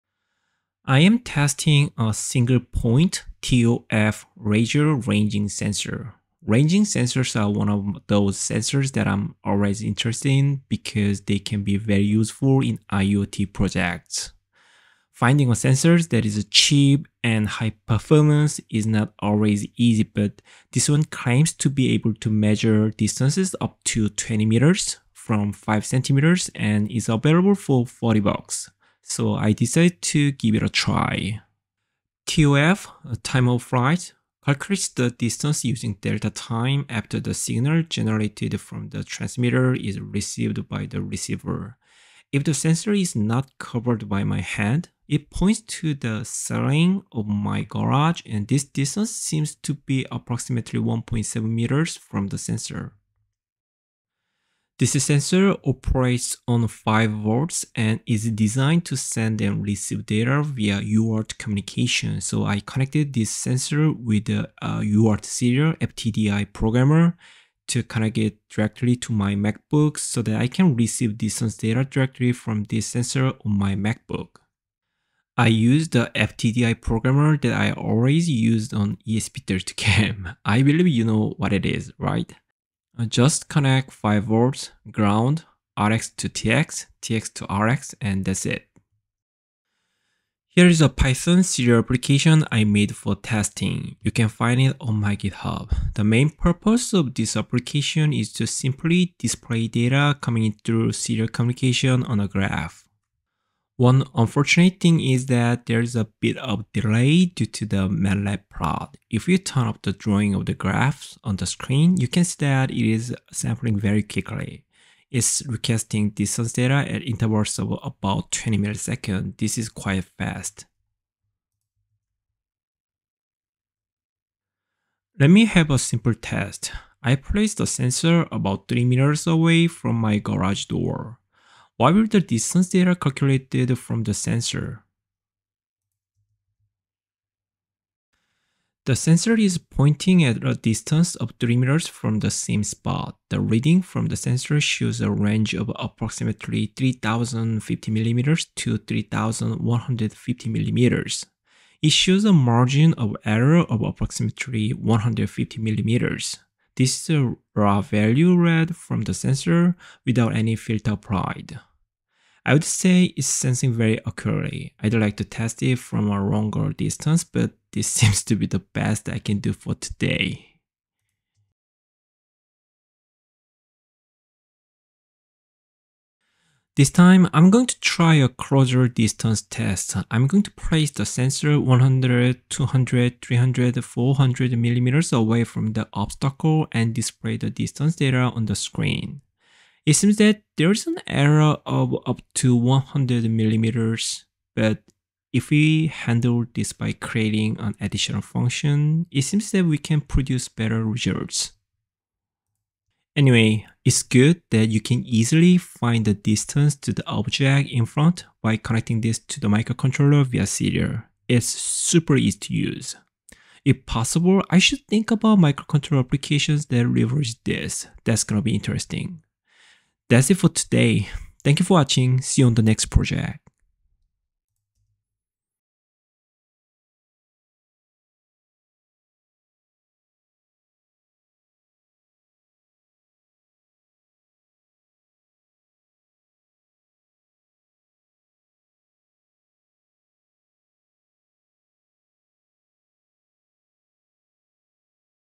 Testing Single-Point TOF Laser Ranging Sensor with Custom Python Serial Program
In this video, I put a Single-point TOF Laser Ranging Sensor to the test using a custom Python serial program I implemented. Watch as I demonstrate how the sensor's distance measurements are captured and visualized in real-time through a Python-based GUI. This video highlights the accuracy and performance of the TOF sensor while showcasing the capabilities of the Python serial communication interface I built.